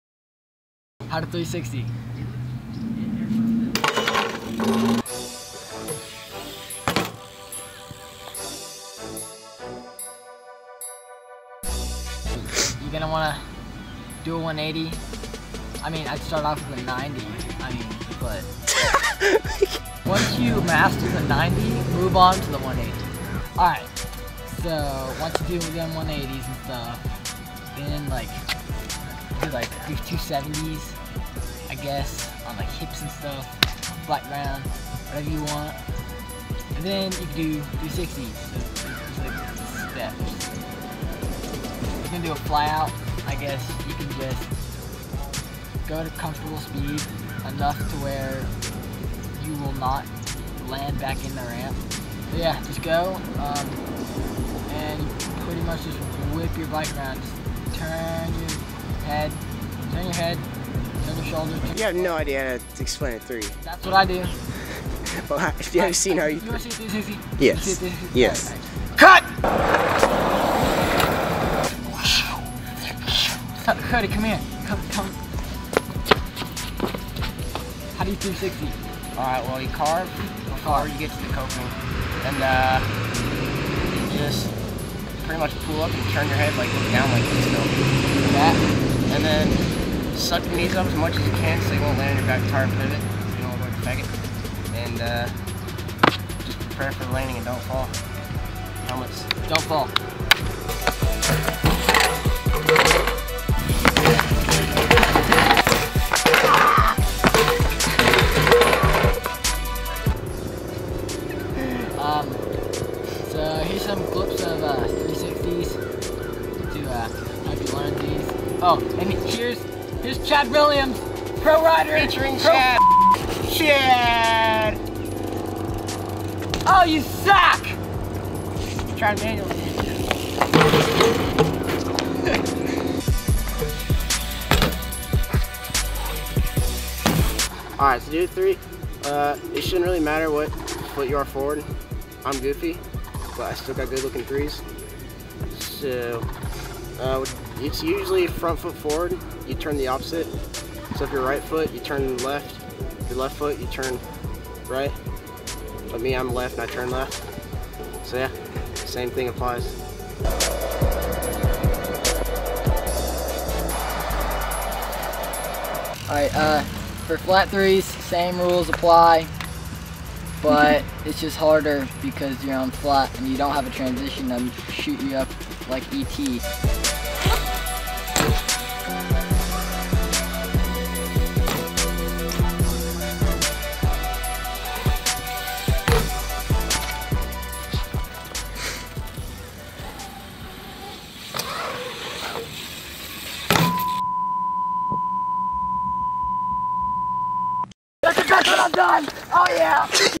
How To 360 BMX
Matt Vogler and Cody Mintel teach you how to 360 on a bmx bike